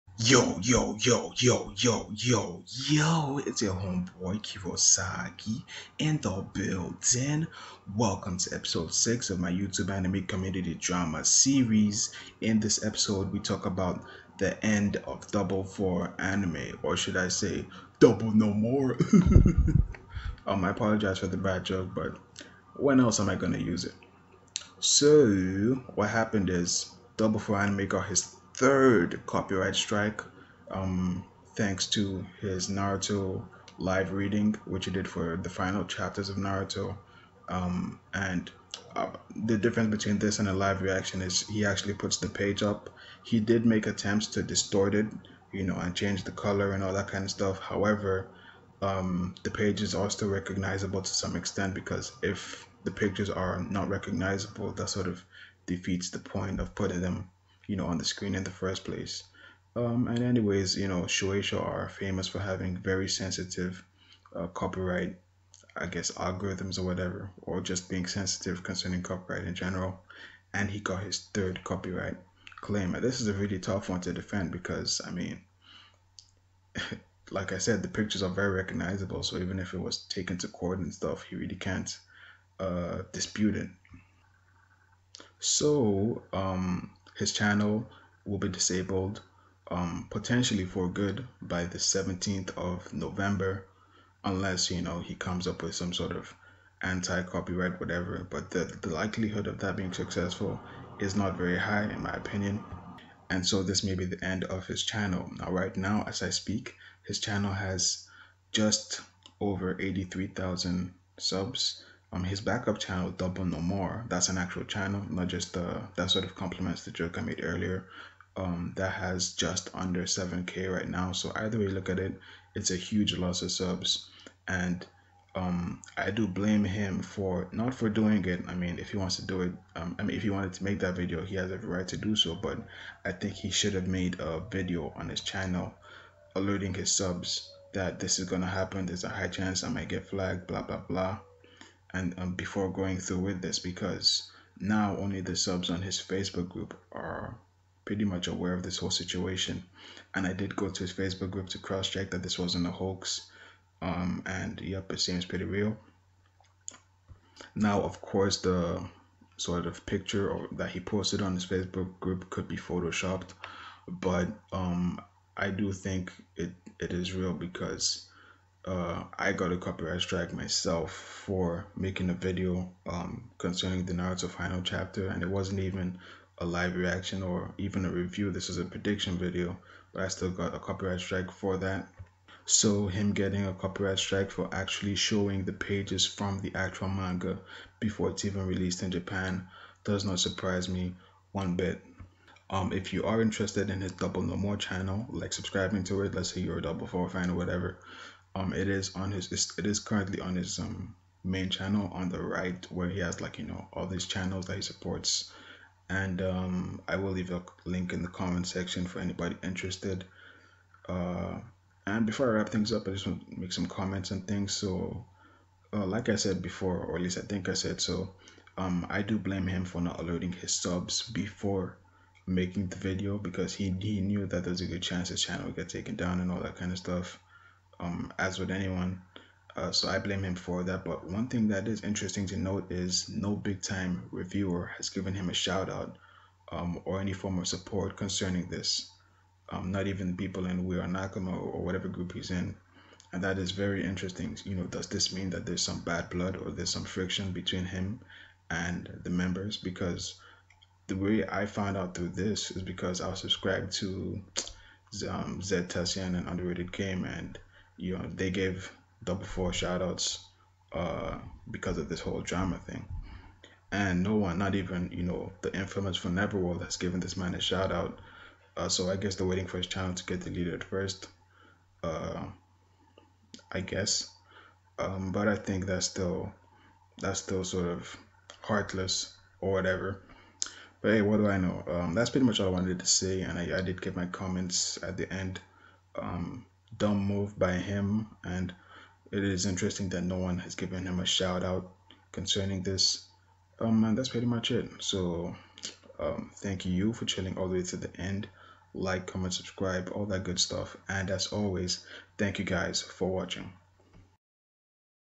YTAC Drama Episode 6: The End of Double4Anime
Link to his backup channel in the comments.
Do you think he is wrong for knowingly putting his channel at risk?
How come none of his "homies" have shown support?